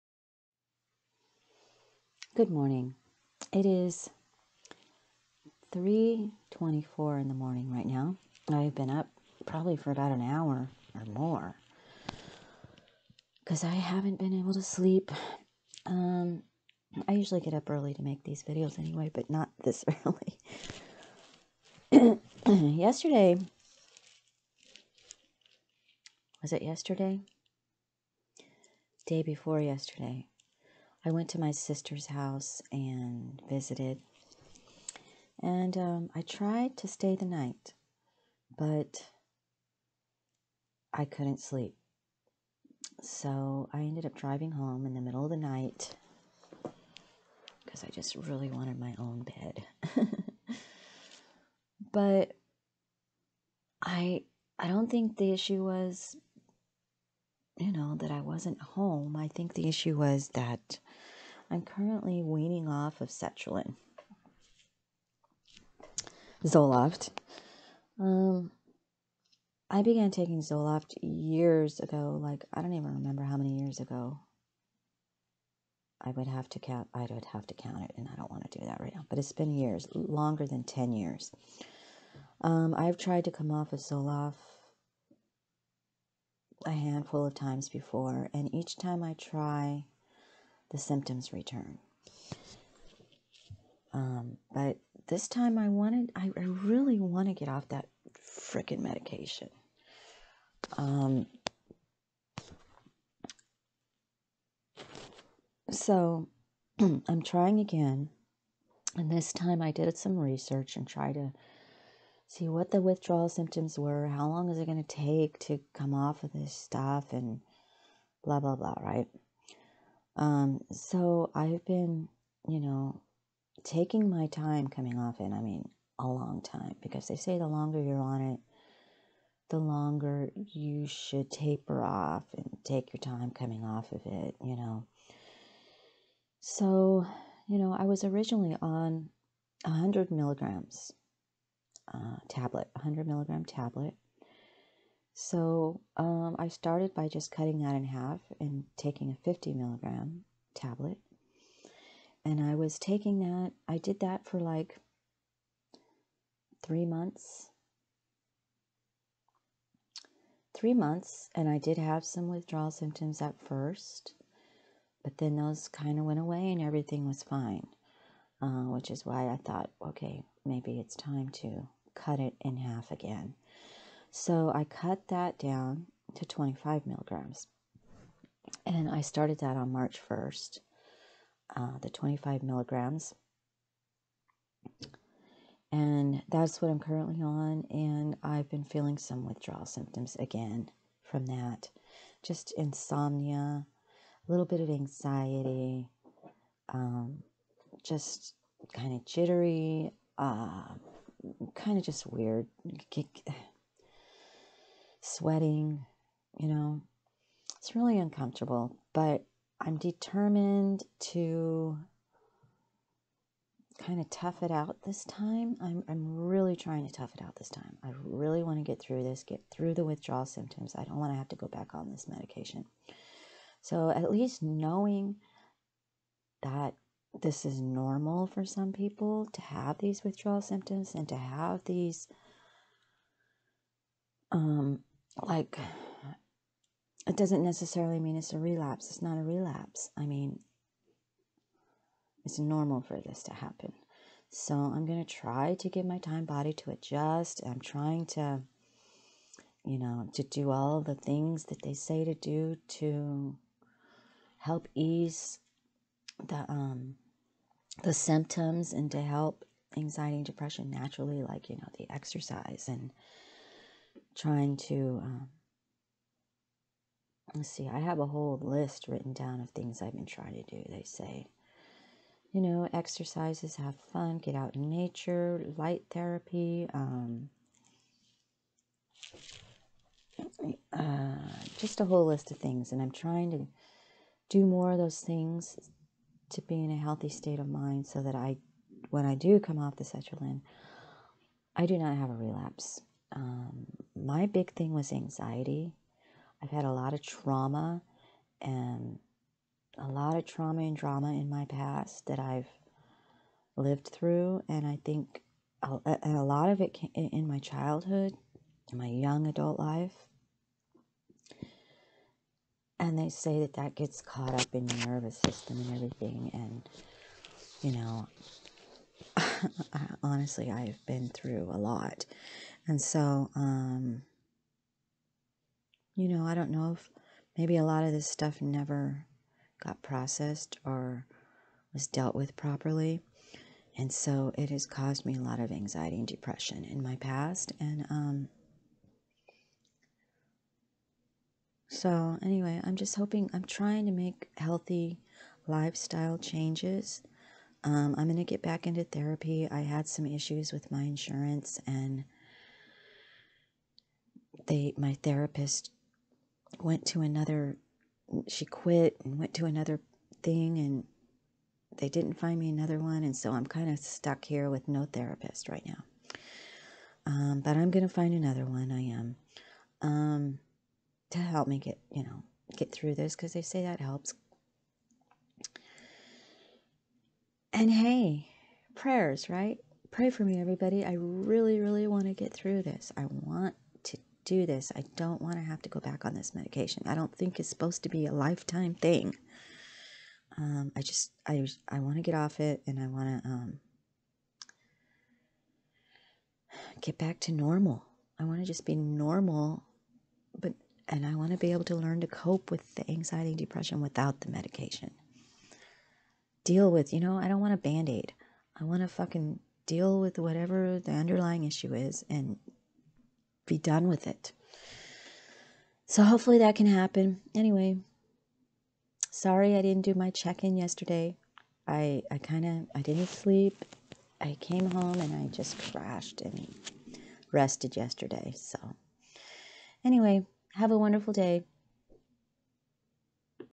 Weaning Off Zoloft
Hello everyone,
Sharing my experience weaning off of sertraline (zoloft).
Have a wonderful day.
Teddy

I Have A Shop! Check it Out!